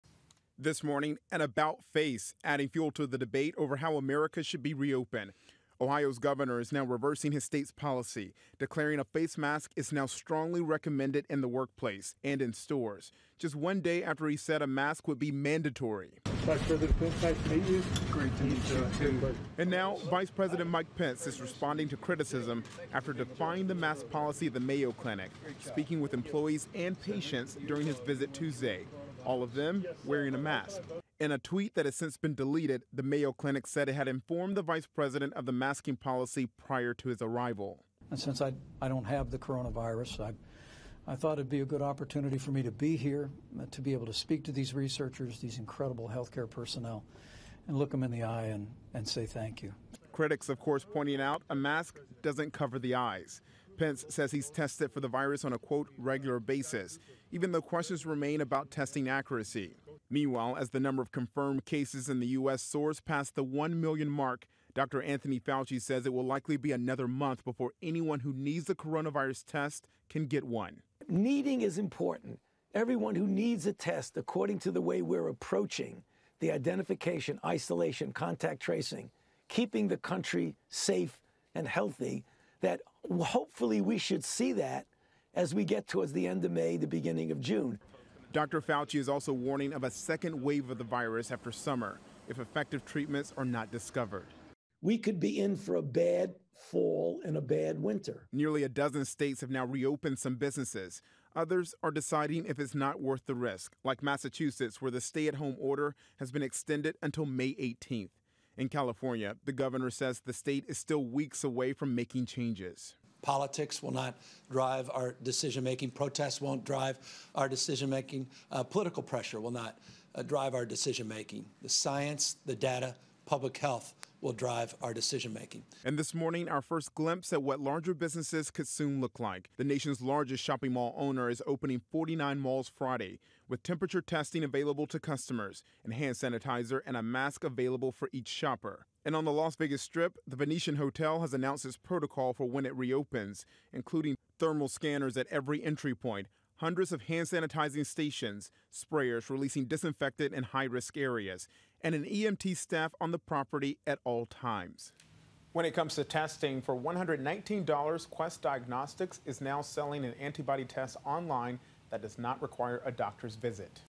Vice President Pence criticized for not wearing mask
This comes as Ohio's governor reverses a policy making it "highly recommended" to wear a mask in the workplace or in stores instead of "mandatory." ABC News' Kenneth Moton reports.